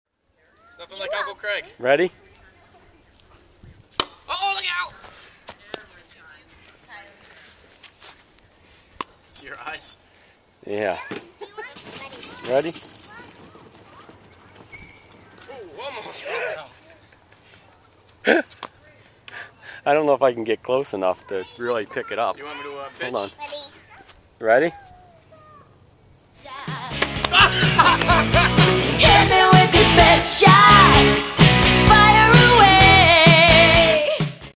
Kate plays wiffle ball.mpg
Kate had been doing great hitting the wiffle ball all afternoon. We noticed that when she was following the ball in and about to swing the bat, her eyes got real huge (then she'd close them after swinging, of course). I used a Flip UltraHD to get a recording of her eyes. Its pretty funny.